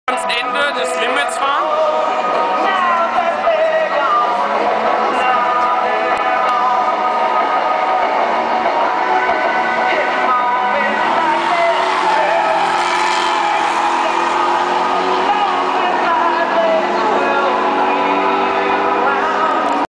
Norbert Haug wird verfolgt vom Audi R8
Norbert Haug in einem Klassiker Benz auf dem Nürburgring! Er wird verfolgt von einem Audi R8 als Renntaxi version!! Wurde aufgenommen bei einem DTM Rennen!